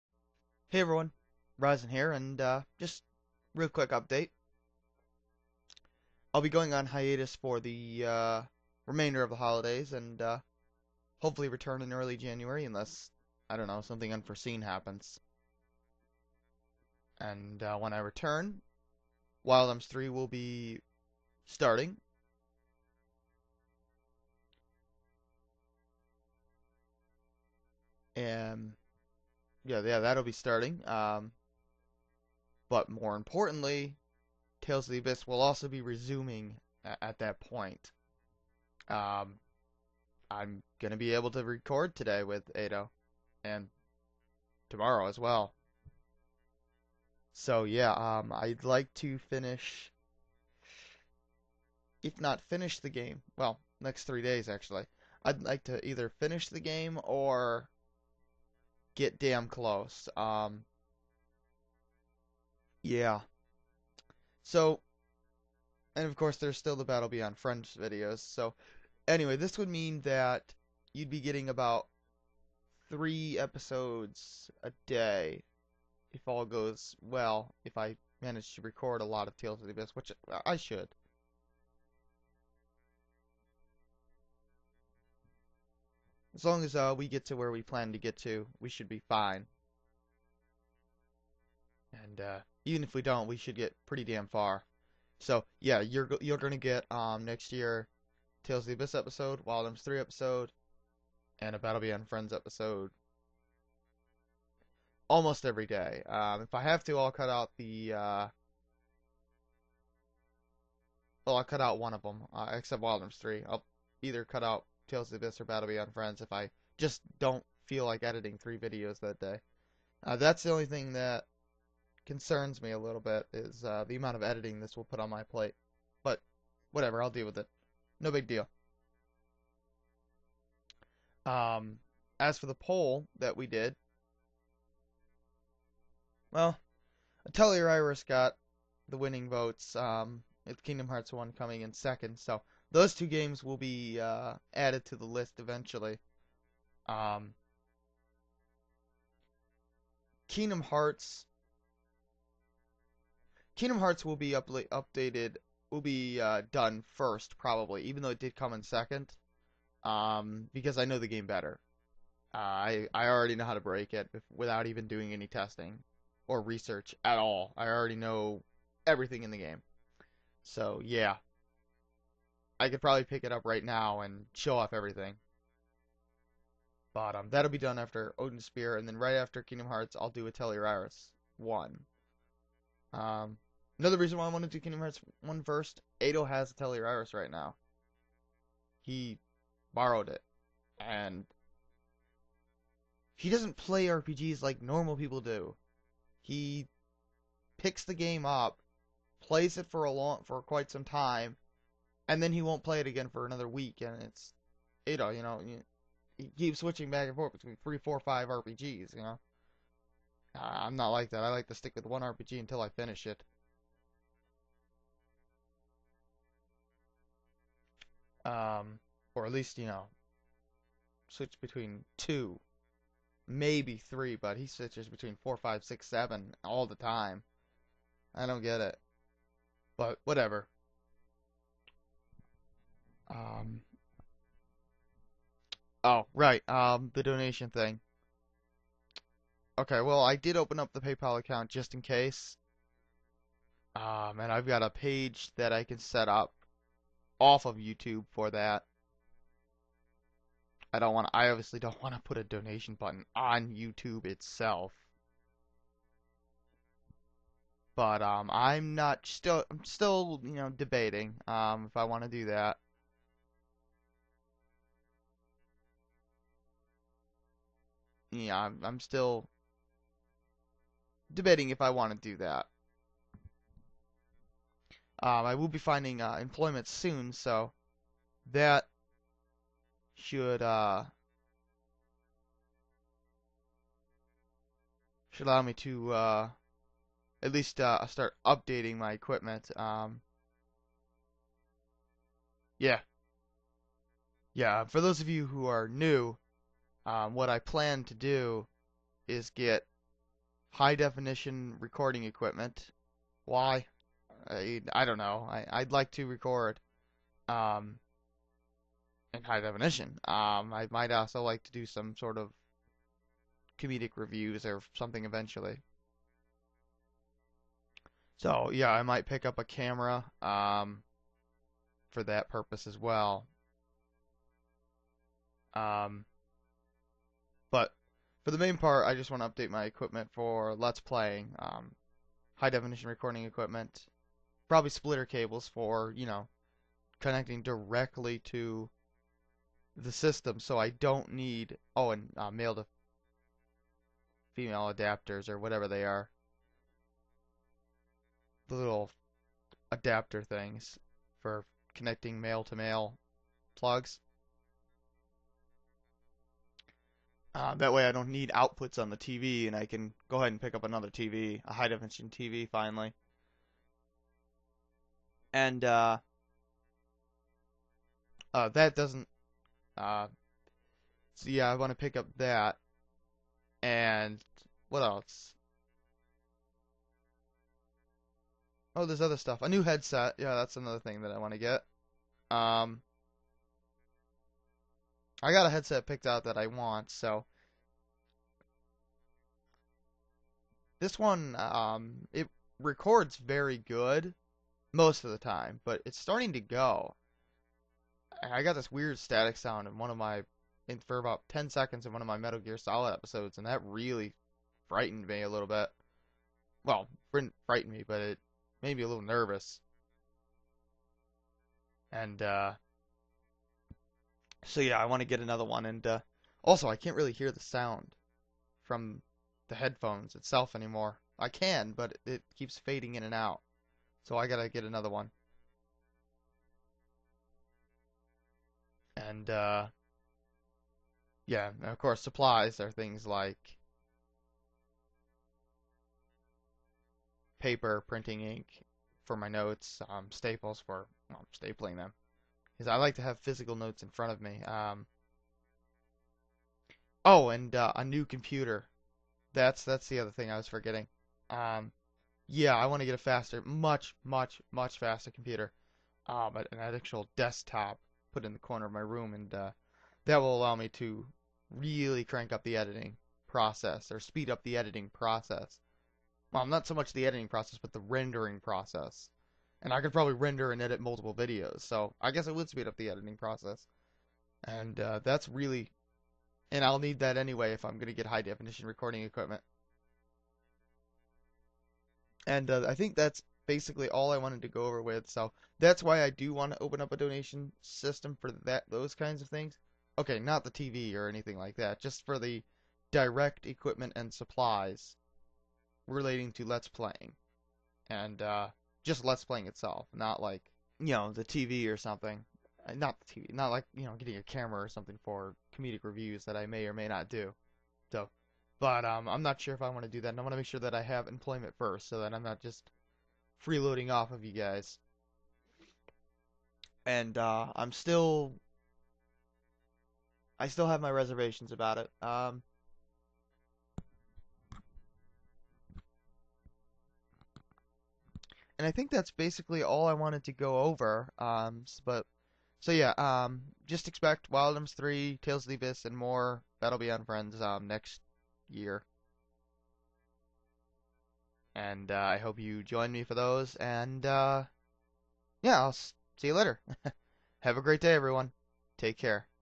Update
Just anothe update video.  I'm going on hiatus, but when I return Tales of the Abyss will be returning and Wild Arms 3 will be starting.